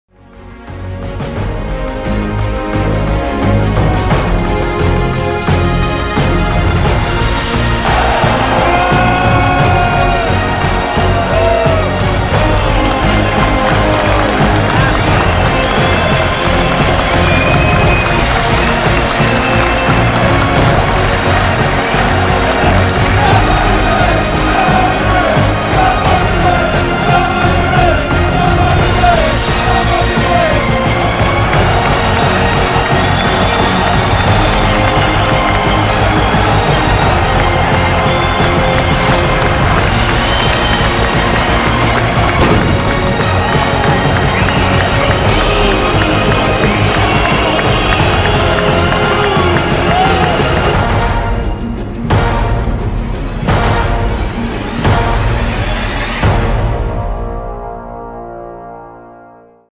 Legendarische tifo R Antwerp FC
Indrukwekkende en legendarische tifo van R Antwerp FC tijdens de wedstrijd R Antwerp FC - Standard Luik